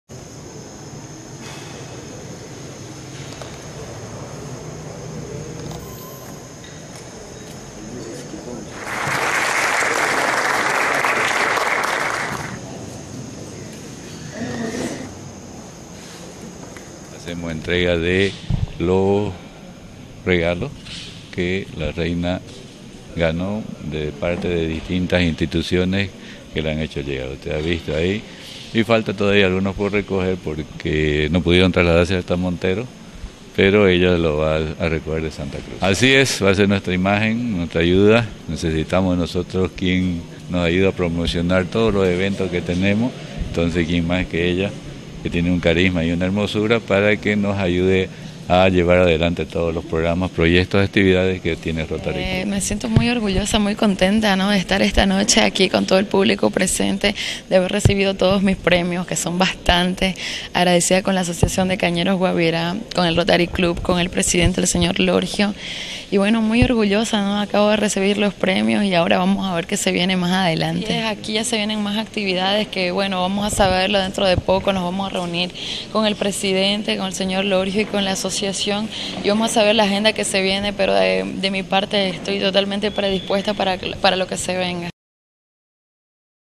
ROTARY ENTREGA DE PREMIOS A LA REINA DE LA CAÑA
la soberana del reinado de la caña Priscila recibió sus premios por sr la mas bella del certamen